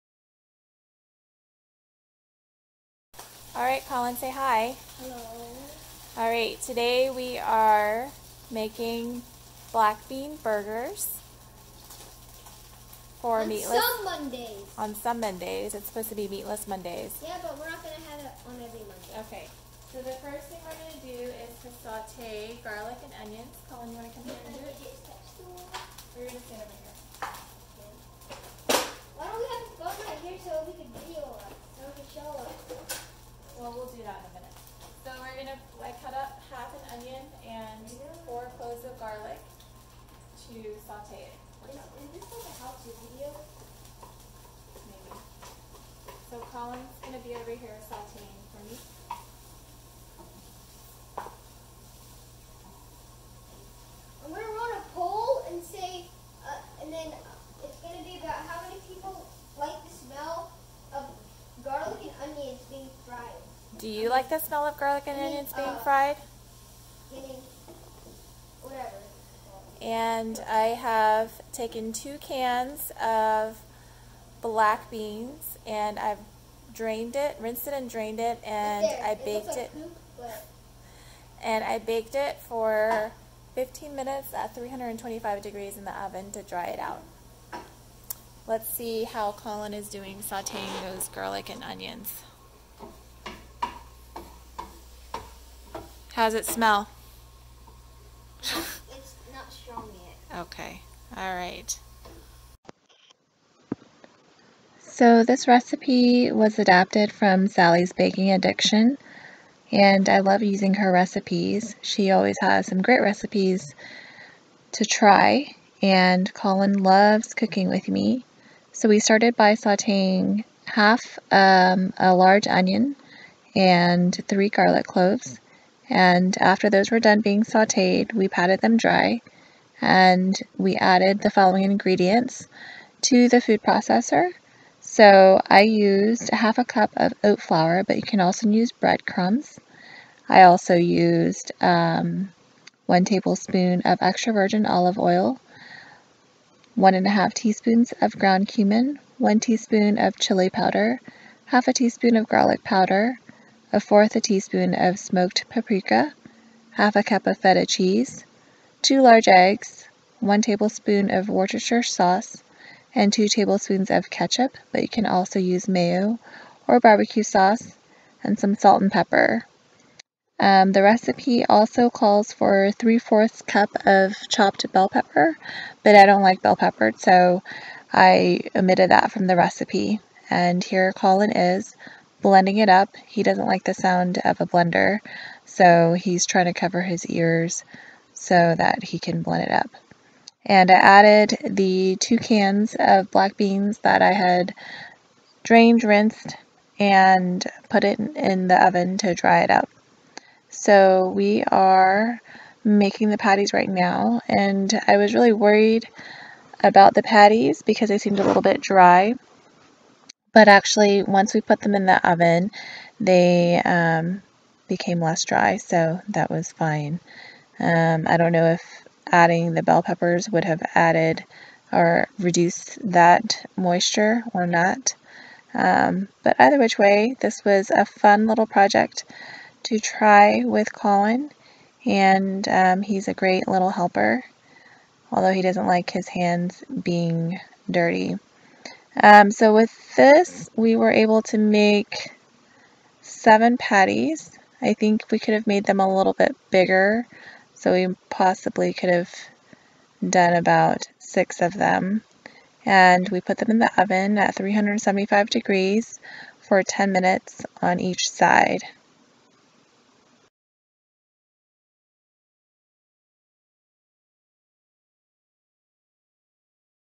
Cooking with Colin
This is our first video! We try a recipe adapted from sallysbakingaddiction.com

We made black bean burgers for dinner on our MeatlessMonday